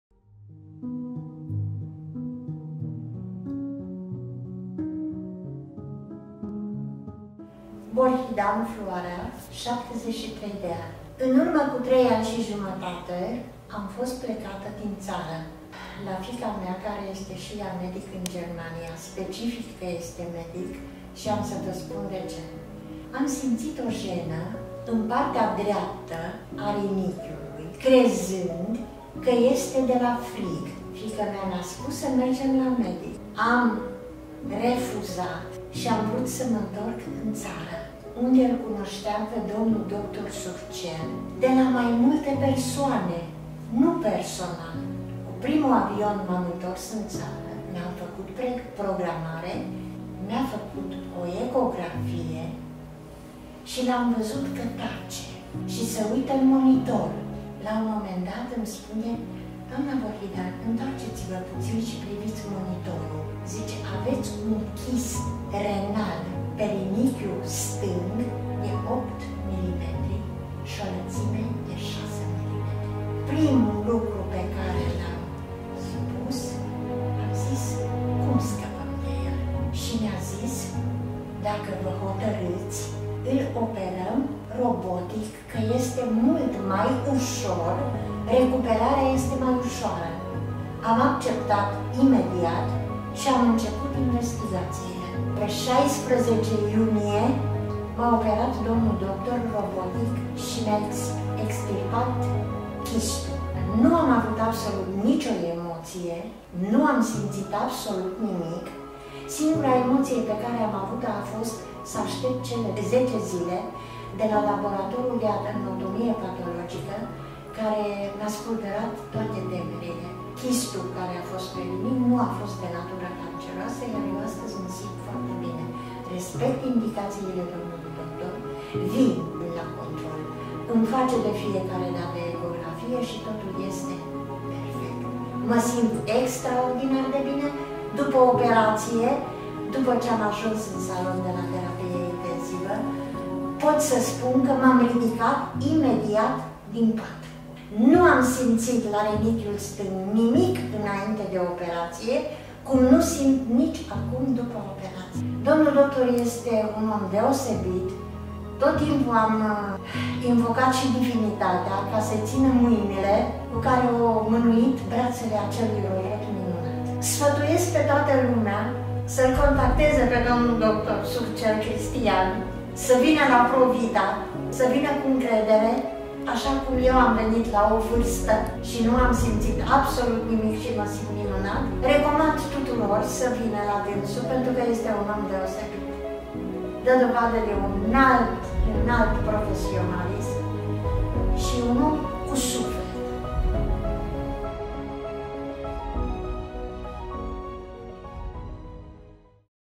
Testimonial pacient chirurgie robotica urologica - conf. dr. Cristian Iulius Surcel - Nefrectomie
"[…] Ma simt extraordinar de bine dupa operatie. Dupa ce am ajuns in salon de la terapie intensiva, pot sa spun ca m-am ridicat (aproape) imediat din pat. [...] Domnul doctor este un om deosebit, tot timpul am invocat si divinitatea ca sa ii tina mainile cu care a manuit bratele acelui robot minunat […]" -  declaratia dnei Borhidan Floarea
Diagnosticata cu un chist pozitionat pe rinichiul stang in dimensiune 8 mm,  procedura medicala a constat in ablatia prin abord robotic executata de Conf. Dr. Cristian Surcel - Chirurg Urolog, medic primar urologie, Doctor în Știinte Medicale.
Satisfactia redobandirii starii de sanatate, revenirea bucuriei de viata au constituit principalii factori motivatori deplin atinsi de medic si echipa medicala, contribuind in acest mod la indeplinirea misiunii in baza careia orice structura medicala (clinica/spital) isi argumenteaza existenta.